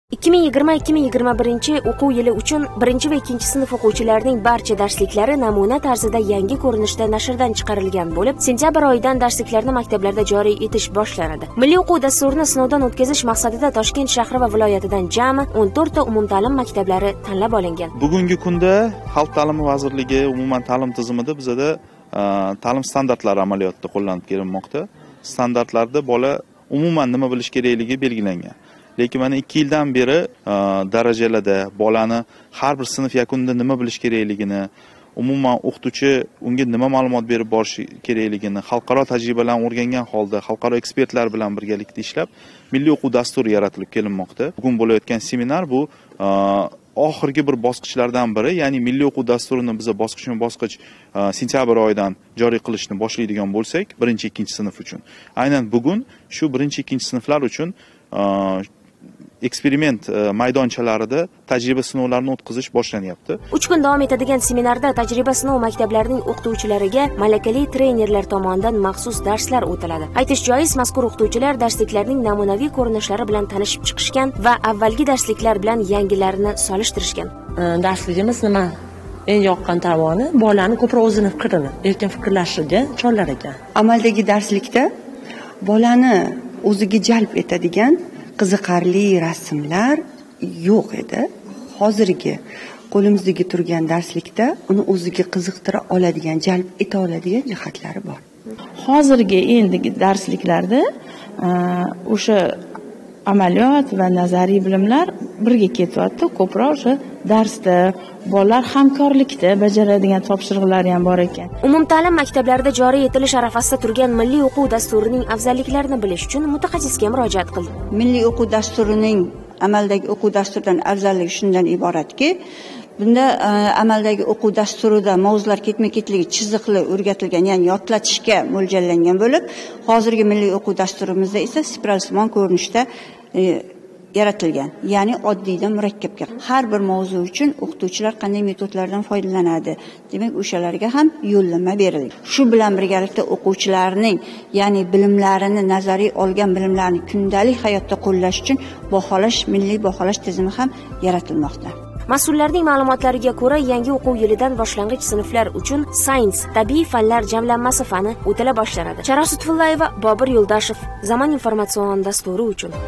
Milliy oʻquv dasturini maktablarda tajriba-sinovdan oʻtkazish jarayonlariga tayyorgarlik boshlandi
Darsliklarni oʻquvchilar qanday qabul qilishini aniqlash maqsadida 1–2-sinf uchun 2021–2022-oʻquv yiliga tayyorlanayotgan yangi, zamonaviy darsliklar ilk bor tayanch maktablarda tajriba-sinovdan oʻtkaziladi.